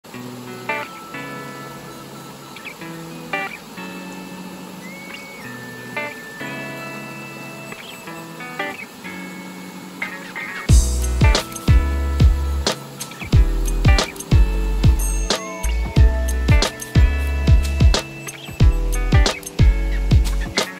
Castor Semi-Looper
A moth caterpillar which was spotted during a trail . I was surprised to see a caterpillar on a very dry looking branch. But when i checked, the branches were just sprouting out new leaves and this guy munched every bit of it!